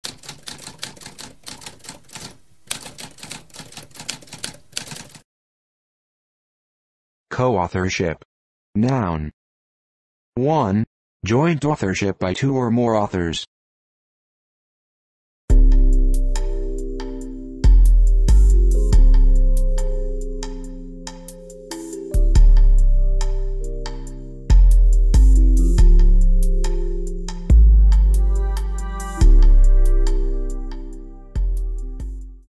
What does coauthorship mean?
A spoken definition of coauthorship.

Intro Sound:
Typewriter - Tamskp